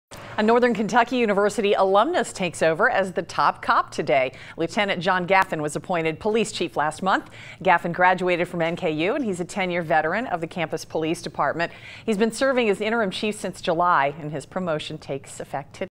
New NKU police chief starts Tuesday
HIGHLAND HEIGHTS, Ky. (WKRC) - A Northern Kentucky University alumnus takes over as police chief of the university Tuesday.

Lieutenant John Gaffin was appointed police chief in December.

Gaffin graduated from NKU and is a 10-year veteran of the campus' police department. He's also been serving as interim chief since July.

His promotion takes effect Tuesday.